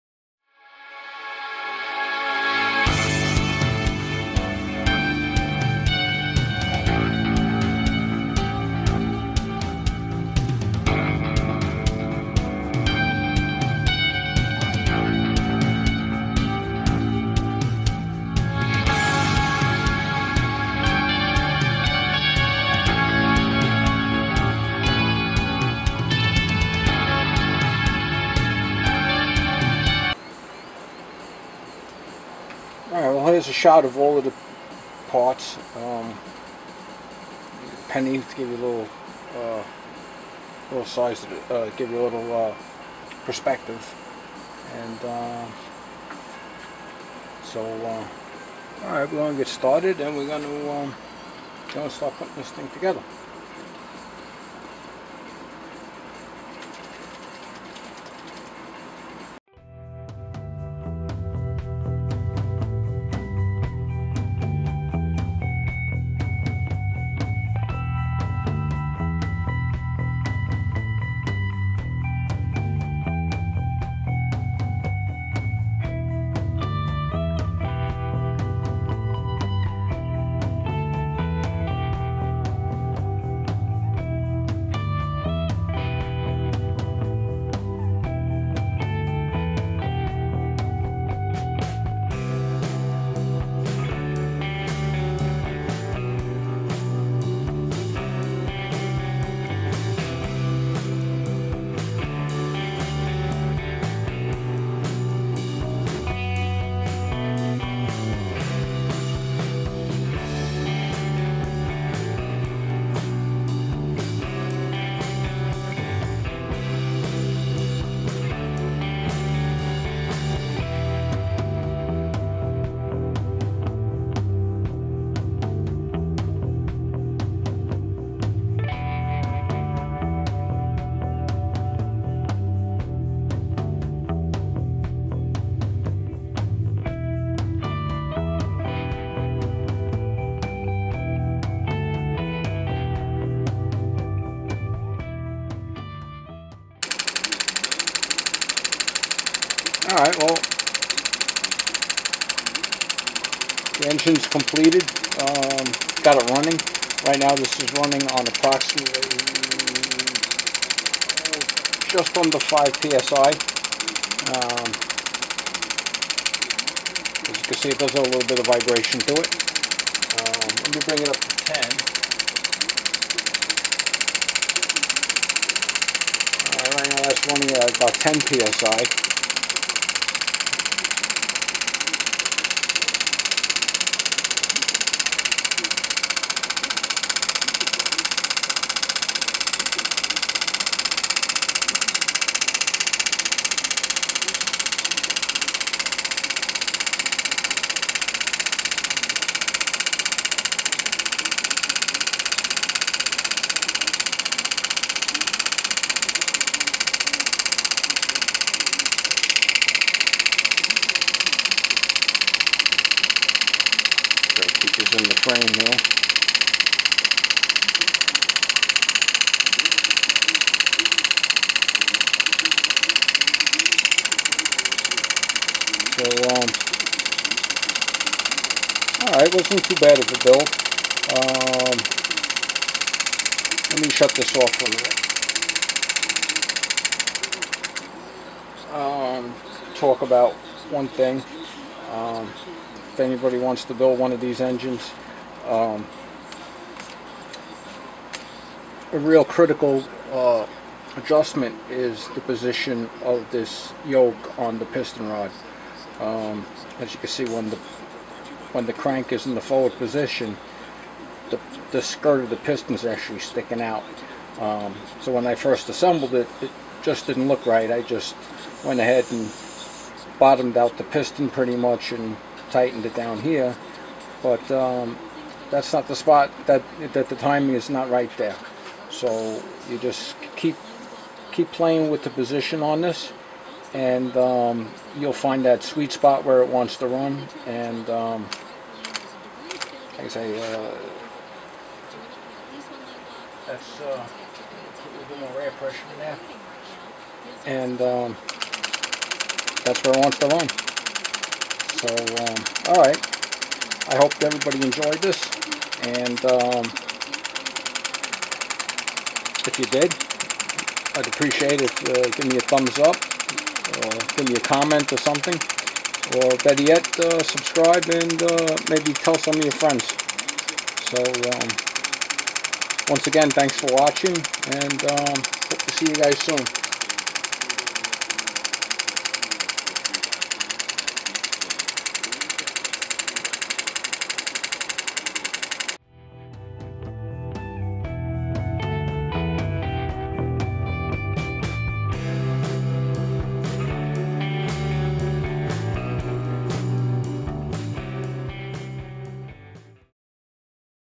Assembling and running Elmer's #8 Scotty Engine
Watch me as I assemble all of the parts I previously made for Elmer's #8 Scotty Engine and then run the engine on compressed air.